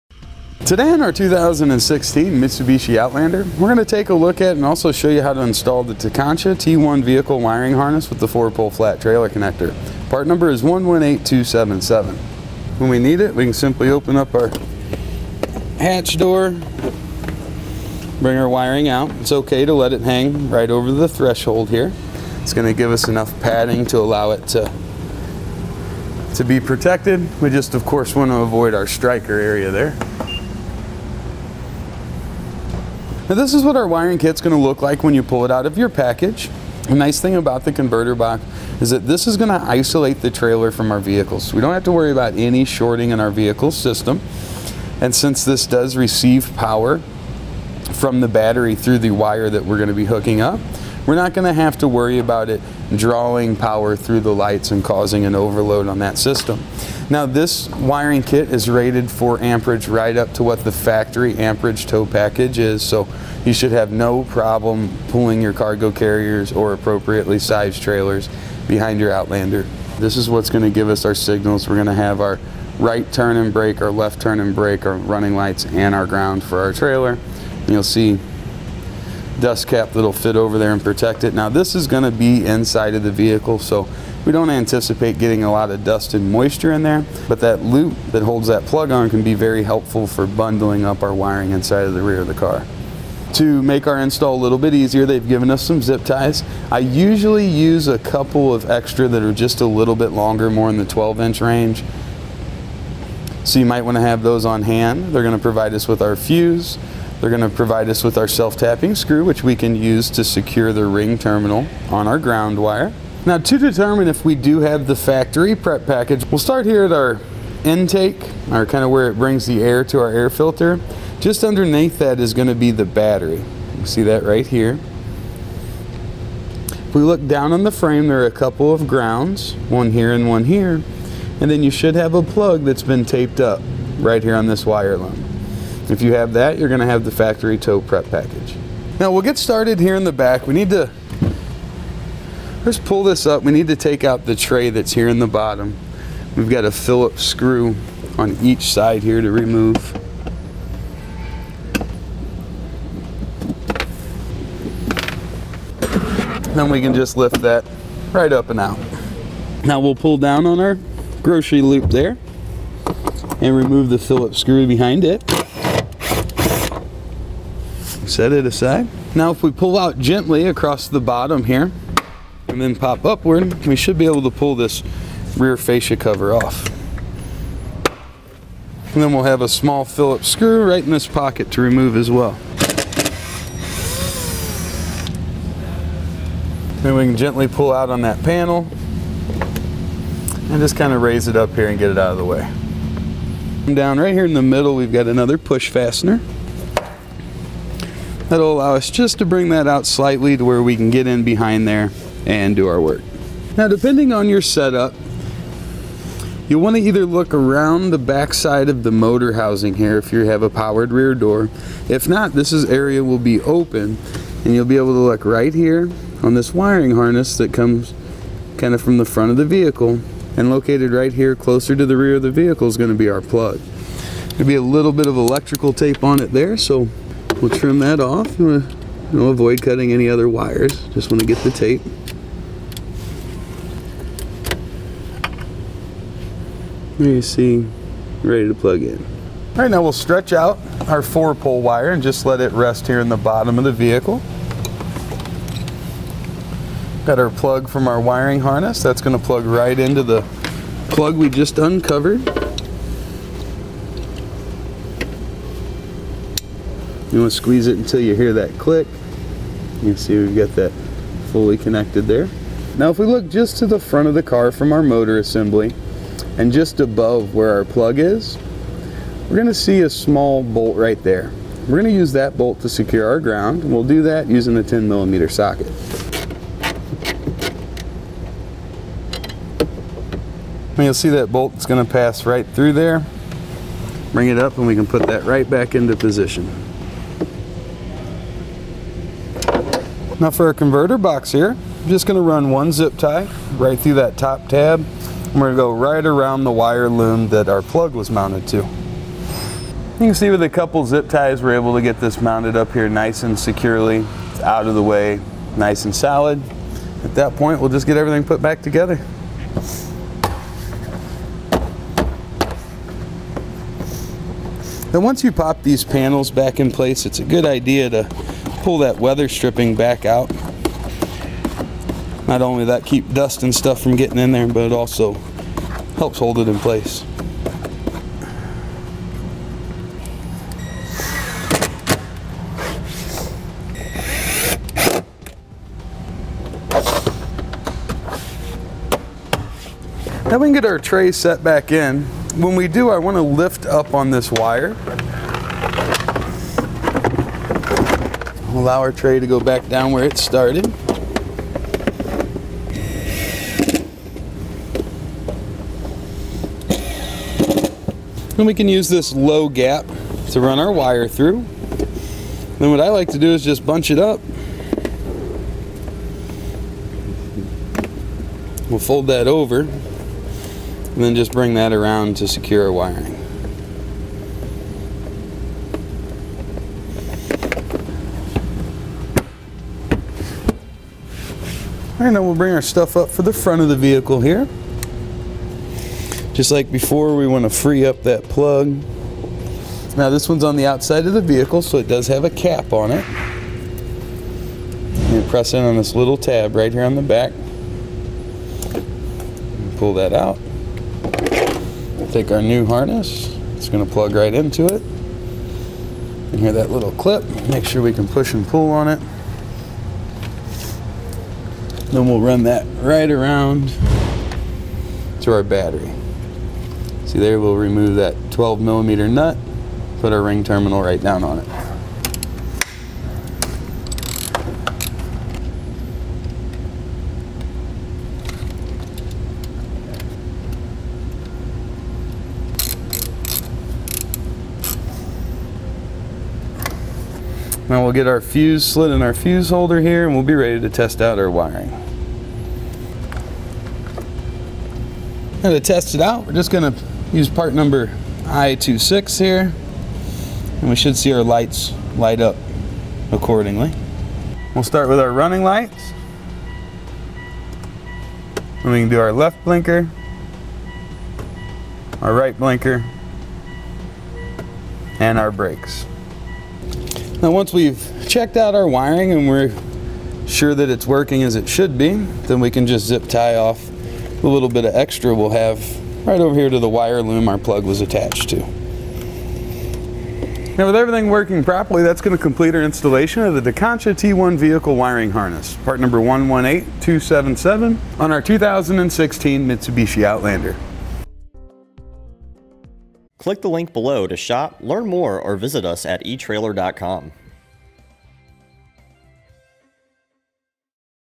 Setting up the T-One Vehicle Wiring Harness for Factory Tow Package on a 2016 Mitsubishi Outlander
Today, on our 2016 Mitsubishi Outlander, we're going to take a look at and also show you how to install the Tekonsha T-One Vehicle Wiring Harness with the 4-Pole Flat Trailer Connector. Part number is 118277. When we need it, we can simply open up our hatch door, bring our wiring out. It's okay to let it hang right over the threshold here. It's going to give us enough padding to allow it to be protected. We just, of course, want to avoid our striker area there. Now, this is what our wiring kit's going to look like when you pull it out of your package.

The nice thing about the converter box is that this is going to isolate the trailer from our vehicle, so we don't have to worry about any shorting in our vehicle system. Since this does receive power from the battery through the wire that we're going to be hooking up, we're not going to have to worry about it drawing power through the lights and causing an overload on that system. Now, this wiring kit is rated for amperage right up to what the factor amperage tow package is, so you should have no problem pulling your cargo carriers or appropriately sized trailers behind your Outlander. This is what's going to give us our signals. We're going to have our right turn and brake, our left turn and brake, our running lights, and our ground for our trailer, and you'll see dust cap that will fit over there and protect it. Now, this is going to be inside of the vehicle, so we don't anticipate getting a lot of dust and moisture in there, but that loop that holds that plug on can be very helpful for bundling up our wiring inside of the rear of the car.

To make our install a little bit easier, they've given us some zip ties. I usually use a couple of extra that are just a little bit longer, more in the 12 inch range, so you might want to have those on hand. They're going to provide us with our fuse. They're going to provide us with our self-tapping screw, which we can use to secure the ring terminal on our ground wire. Now, to determine if we do have the factory prep package, we'll start here at our intake, our kind of where it brings the air to our air filter. Just underneath that is going to be the battery.

You see that right here. If we look down on the frame, there are a couple of grounds, one here and one here, and then you should have a plug that's been taped up right here on this wire line. If you have that, you're going to have the factory tow prep package. Now, we'll get started here in the back. We need to, just pull this up. We need to take out the tray that's here in the bottom.

We've got a Phillips screw on each side here to remove. Now, we can just lift that right up and out. Now, we'll pull down on our grocery loop there and remove the Phillips screw behind it and set it aside. Now, if we pull out gently across the bottom here and then pop upward, we should be able to pull this rear fascia cover off. Then, we'll have a small Phillips screw right in this pocket to remove, as well. Then, we can gently pull out on that panel and just kind of raise it up here and get it out of the way. Down right here in the middle, we've got another push fastener. That will allow us just to bring that out slightly to where we can get in behind there and do our work. Now, depending on your setup, you want to either look around the back side of the motor housing here if you have a powered rear door. If not, this's area will be open, and you'll be able to look right here on this wiring harness that comes kind of from the front of the vehicle, and located right here, closer to the rear of the vehicle, is going to be our plug. Could be a little bit of electrical tape on it there, so we'll trim that off. You want to avoid cutting any other wires. Just want to get the tape. What are you seeing, ready to plug in. All right. Now, we'll stretch out our 4-pole wire and just let it rest here in the bottom of the vehicle. Got our plug from our wiring harness. That's going to plug right into the plug we just uncovered. You want to squeeze it until you hear that click. You can see, we've got that fully connected there. Now, if we look just to the front of the car from our motor assembly and just above where our plug is, we're going to see a small bolt right there. We're going to use that bolt to secure our ground, and we'll do that using the 10 millimeter socket. Now, you'll see that bolt's going to pass right through there. Bring it up, and we can put that right back into position. Now, for our converter box here, I'm just going to run one zip tie right through that top tab, and we're going to go right around the wire loom that ou